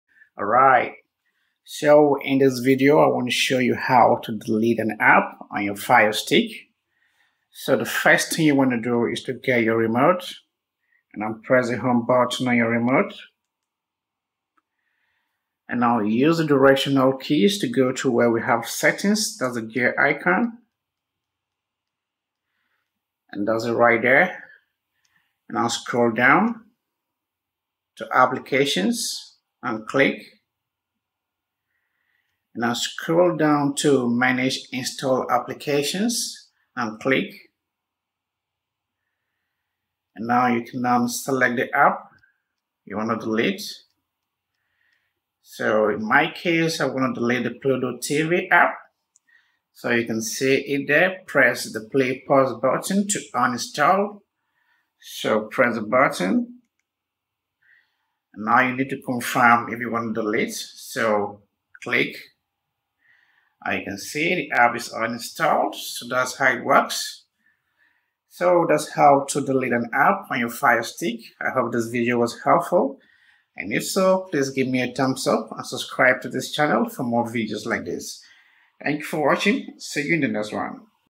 How to Delete Apps on Firestick
If you're not sure how to delete (uninstall) an app on your Amazon Fire TV Stick, this video is for you.

In this video, you'll learn how to delete apps on your Fire TV Stick in seconds.

Keep in mind that some apps on your Firestick can't be deleted. These are pre-installed apps (system apps) that come with the Fire TV Stick 4K.